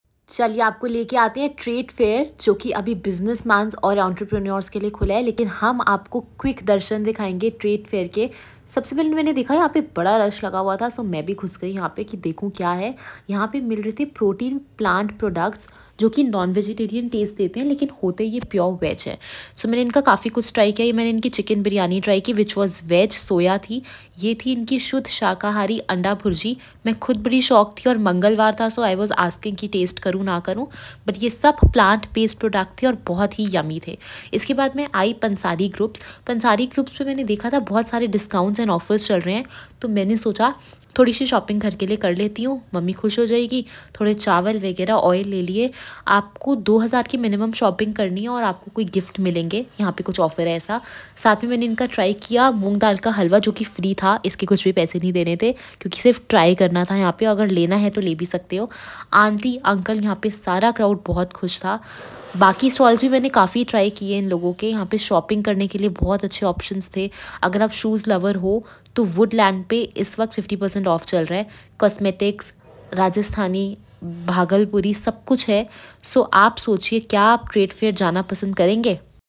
Trade Fair 2022 Pragati Maidan Tour Vlog
Trade fair, also known as trade show, trade exhibition, or trade exposition, is an exhibition organized so that companies in a specific industry can showcase and demonstrate their latest products and services, meet with industry partners and customers, study activities of rivals, and examine recent market trends and opportunities.

Tour trade fair 2022 pragati maidan